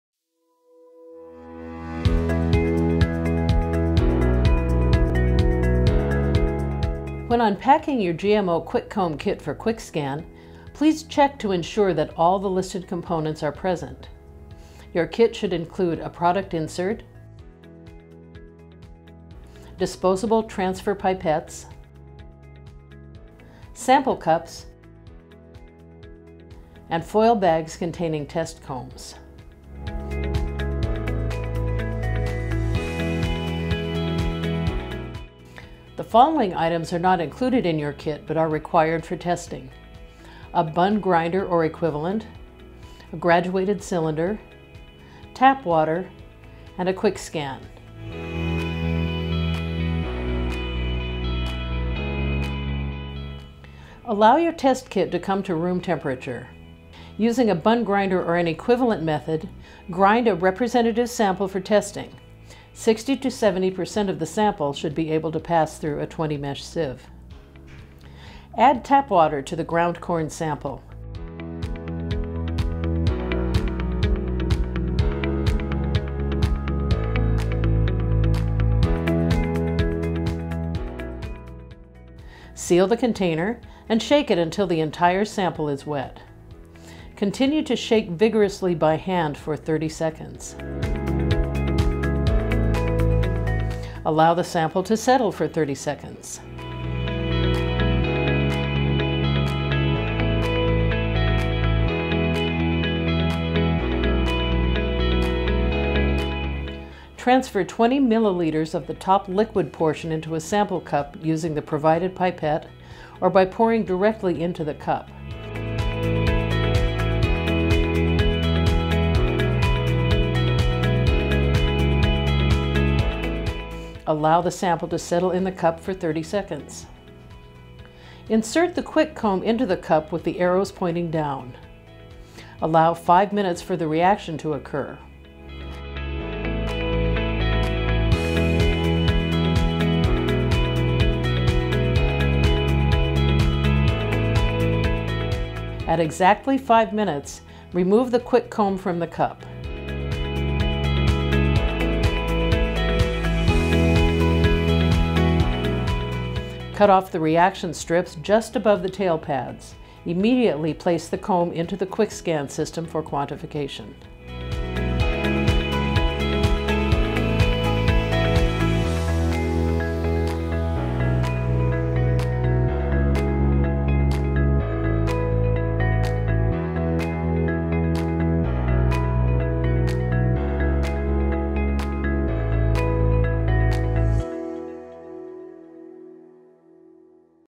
Rapid GMO Testing Corn
How to test ground corn for GMOs using the EnviroLogix QuickComb GMO testing kit.